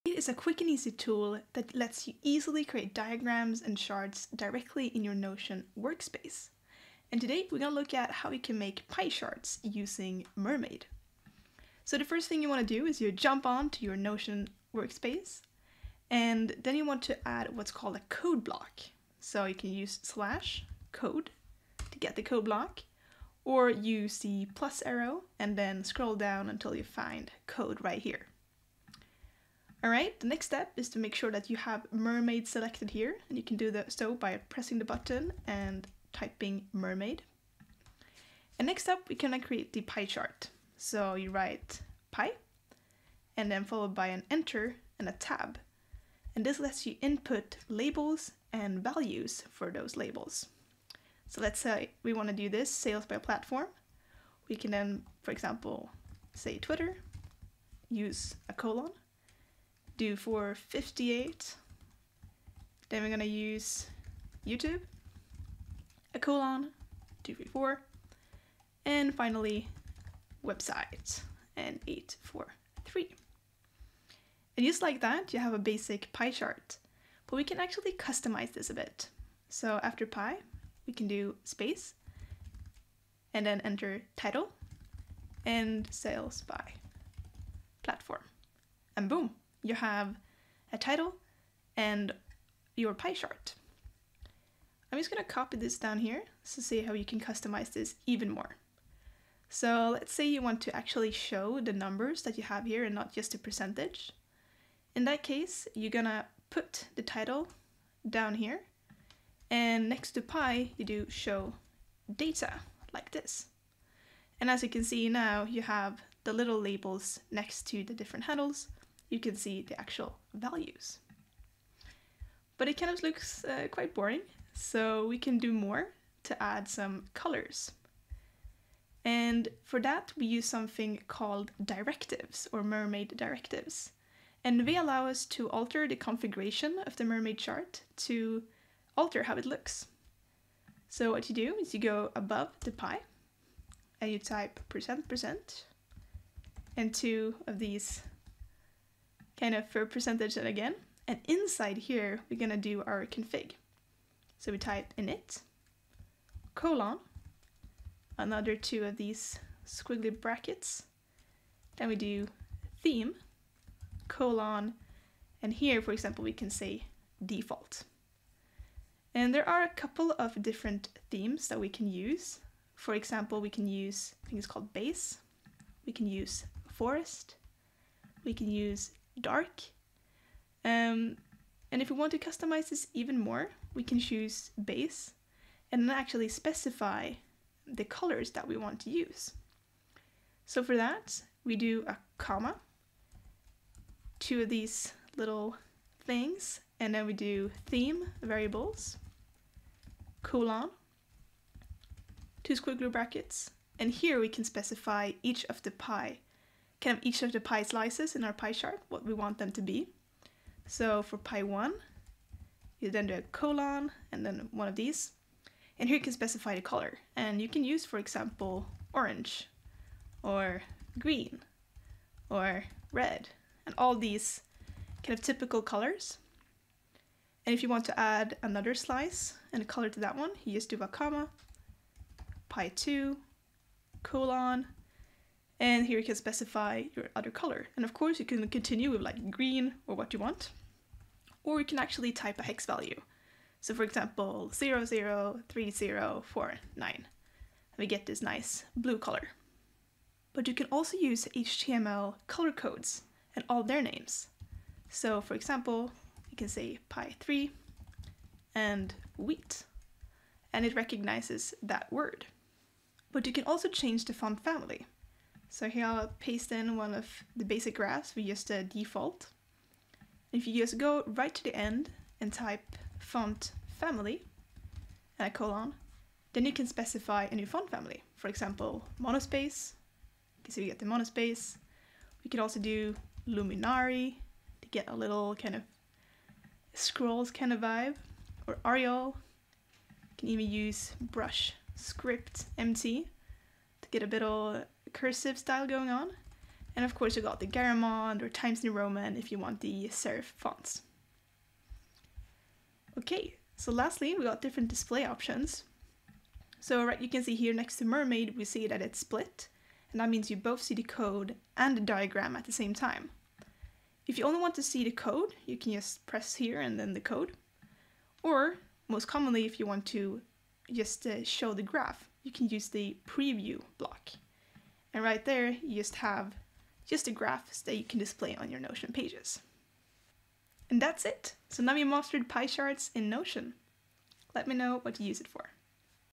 Create Piecharts Directly In Notion Using Mermaid
📒 CREATE PIECHARTS DIRECTLY IN NOTION 📒

Mermaid is a quick-and-easy tool that allows you to embed diagrams and charts directly in your Notion Workspace.

In this video I teach you everything you need to know to get started with Mermaid Piecharts!

Chapters:
00:00 Intro
00:15 Basic Piecharts
01:20 Show Data
02:05 Custom Colors
04:22 Change Fonts
05:02 Display Options
05:30 Outro

*Sources Used*
- Mermaid Website
- Mermaid Piechart Documentation
- Mermaid Directives Documentation
- HTML Color Codes: